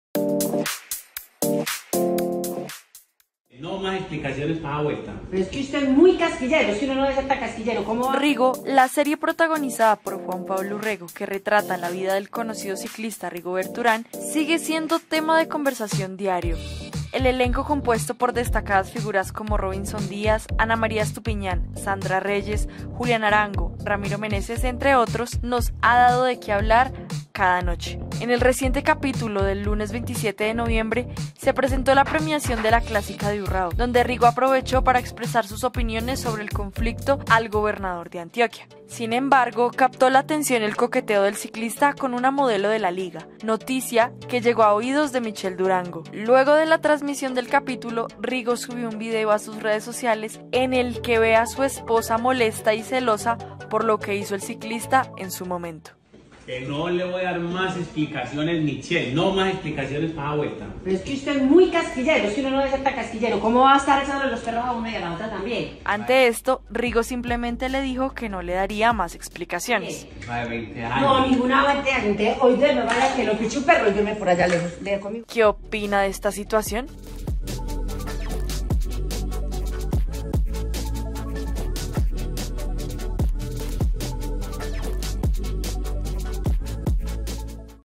Escena de celos a Rigo por capítulo de la novela
Rigo compartió con sus seguidores el reclamo que le hizo su esposa Michelle por una escena de la novela del Canal RCN. Acá le contamos lo que pasó

¿Qué opina de esta situación?